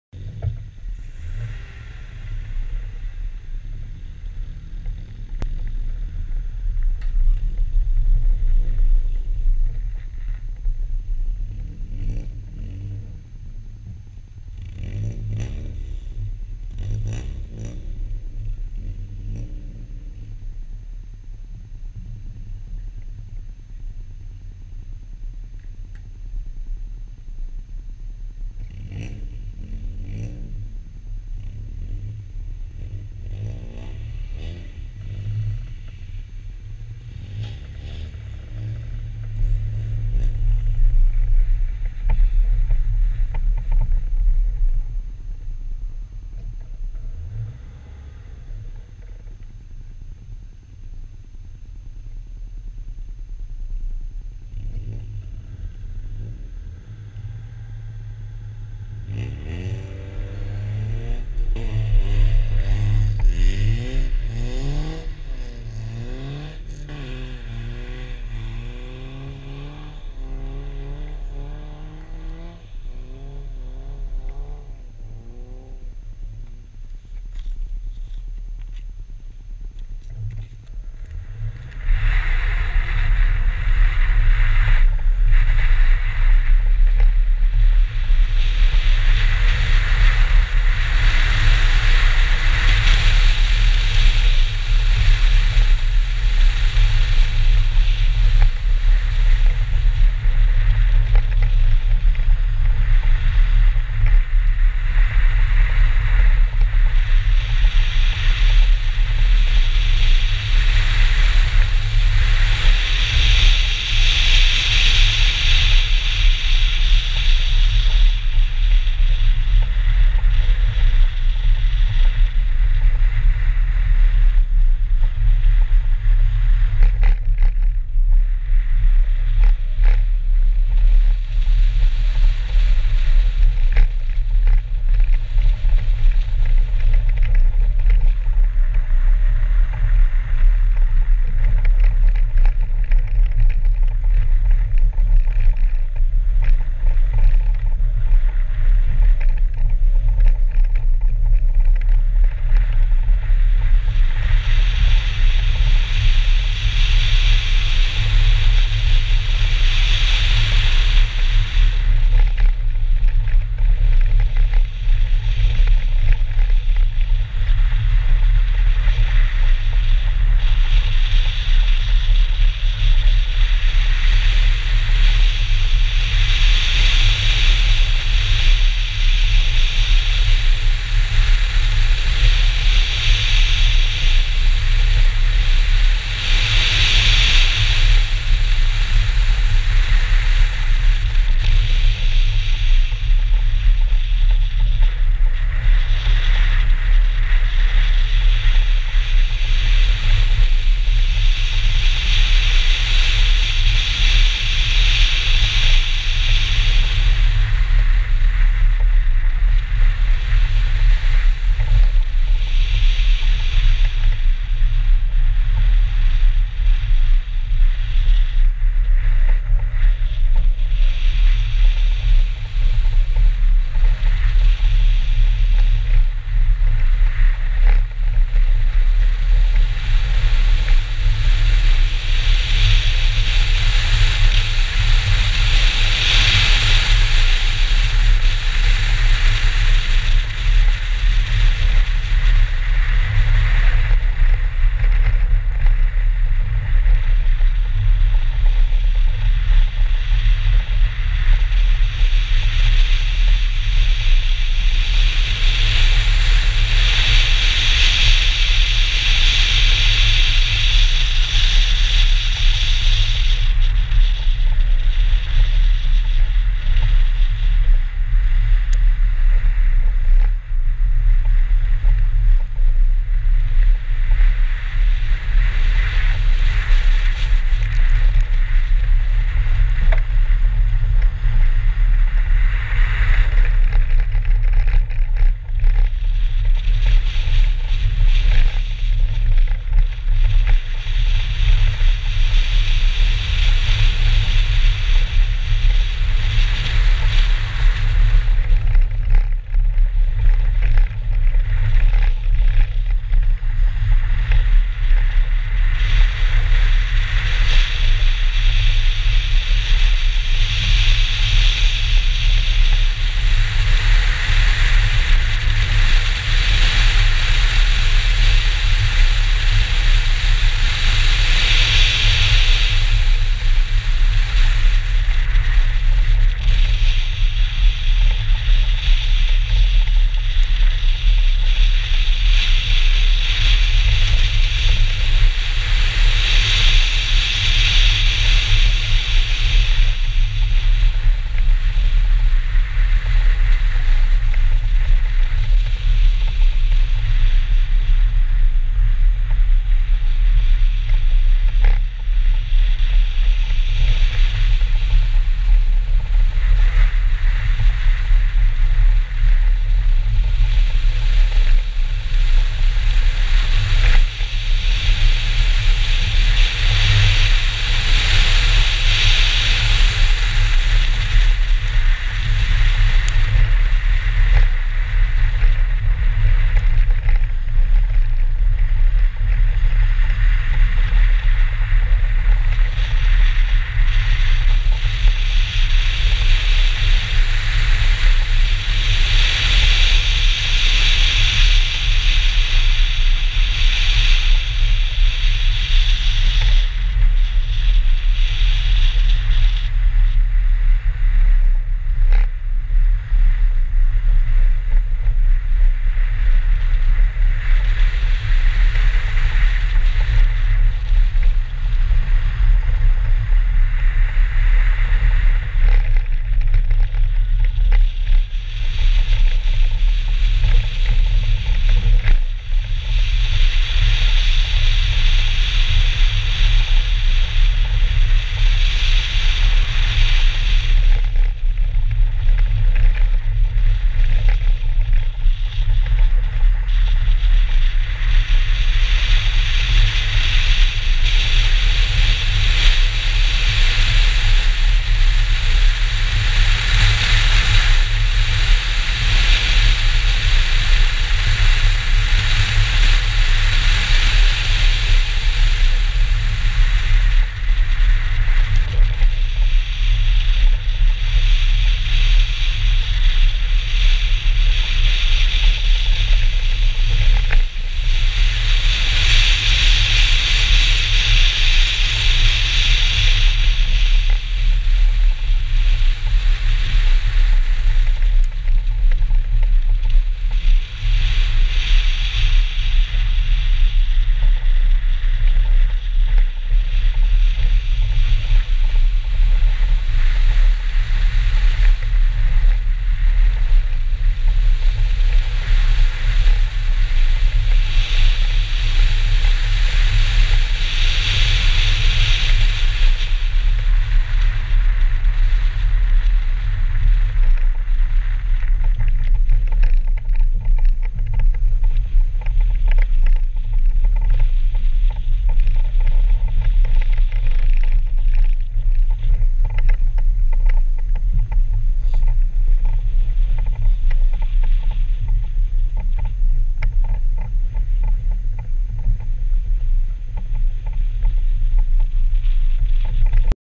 AG Cup. Hyreda 2015. MSFAN. 3 race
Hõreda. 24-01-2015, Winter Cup
MSFAN - BMW320. 3. katse